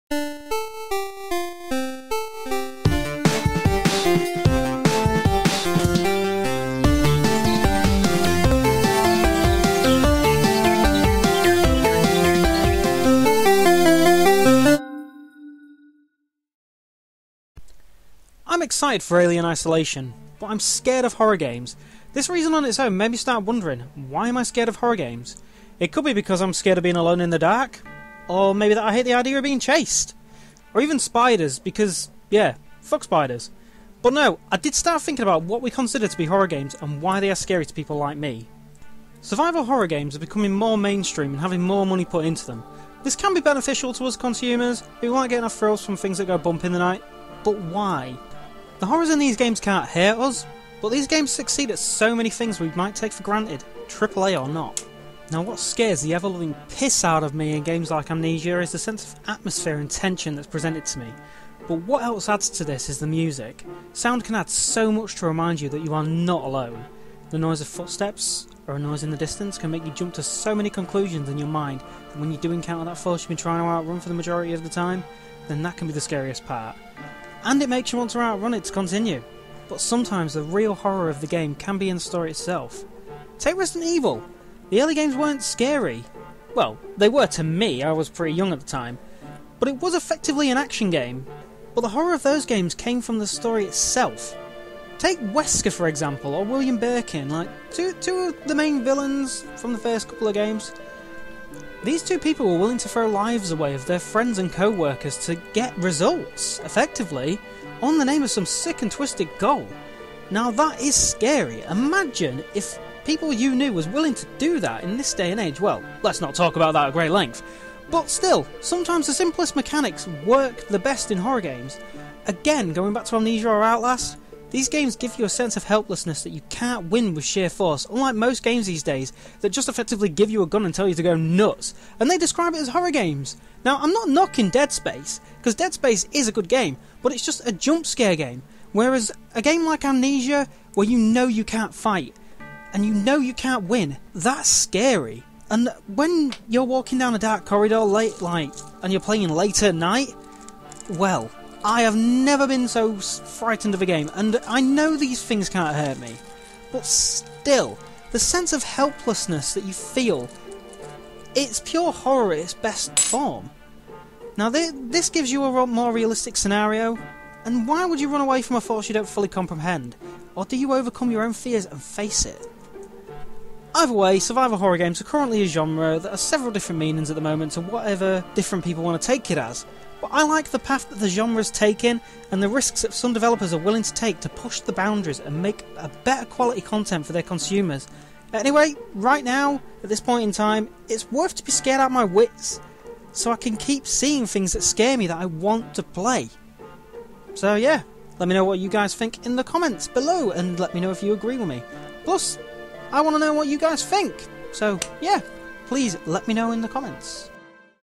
PAUSE MENU - Why are scary games scary?
This week im discussing what might make scary games, well... scary